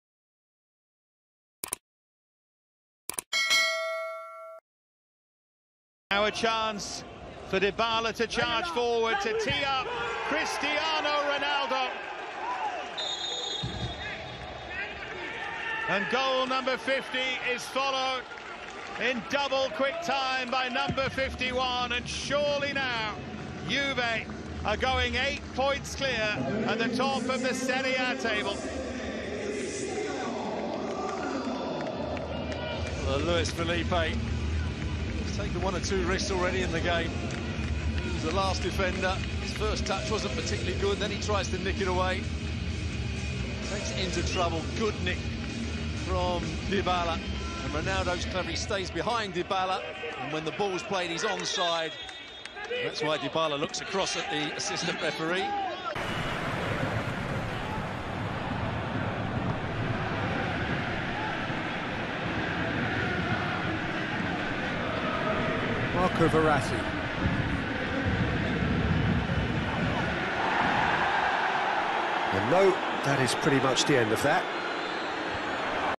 റൊണാൾഡോ ന്യൂ റെക്കോർഡ് /
Ronaldo becomes first player to score 50 Premier League, La Liga and Serie A goals